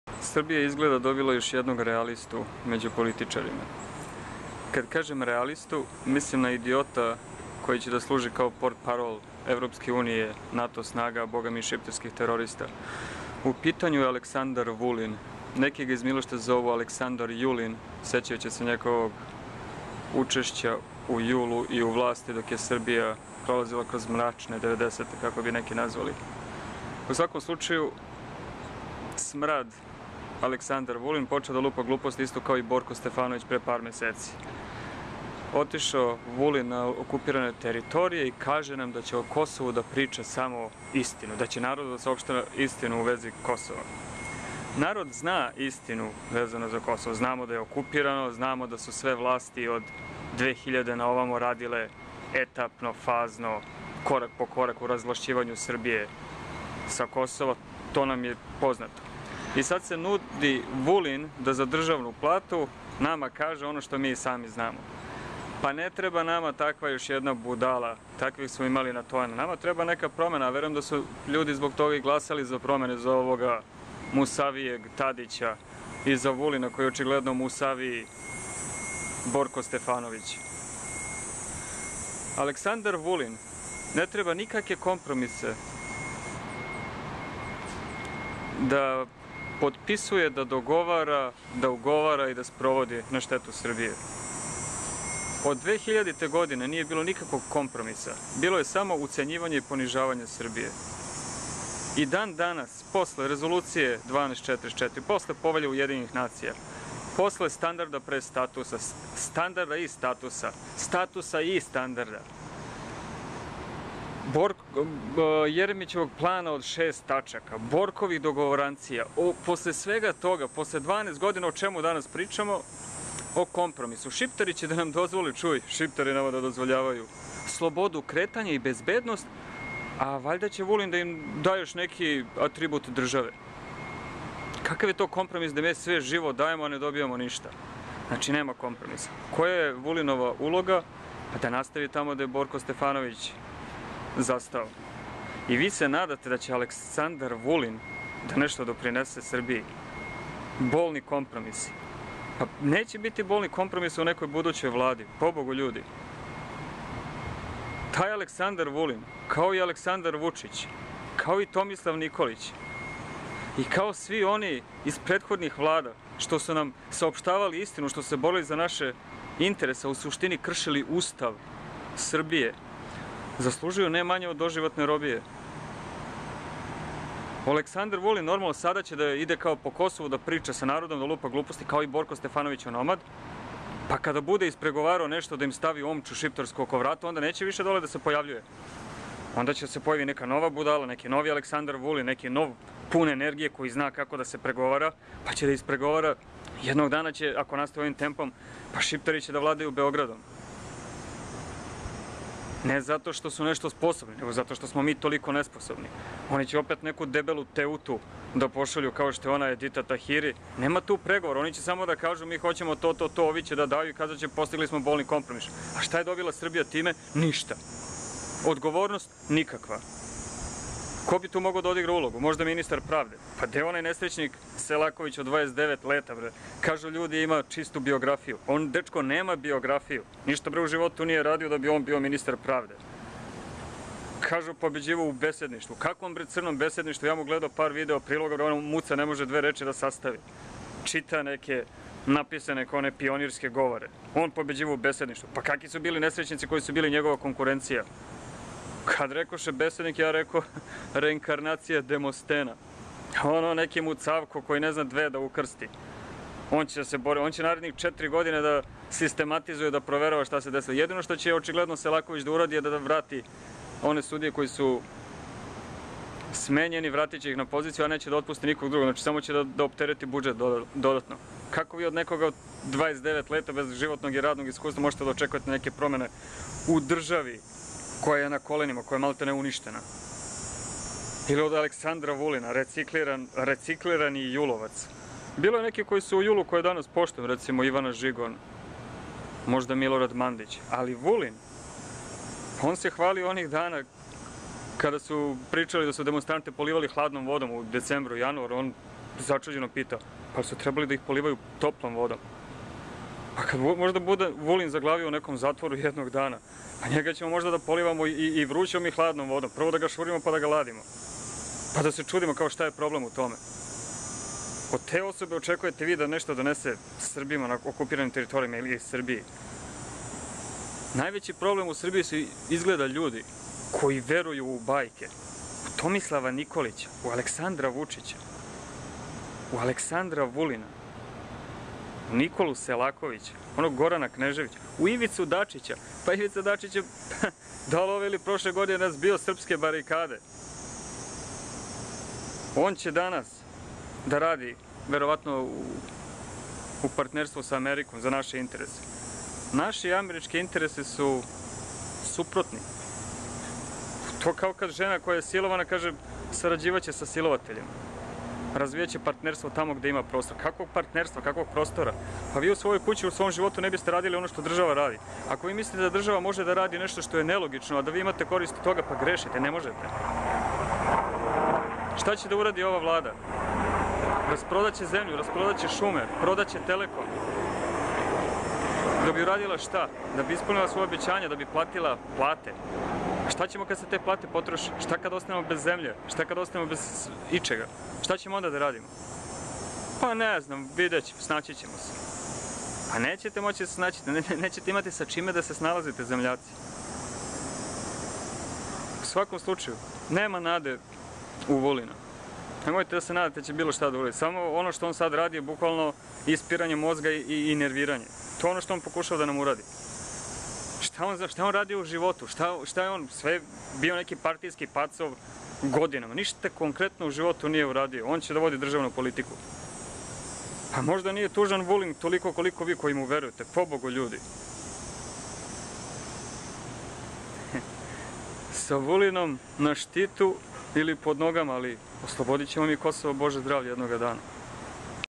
Александар Вулин Мусавији Борко Стефановић
Смањује се сумња да ће Александар Вулин да буде мусавија варијанта Борка Стефановића..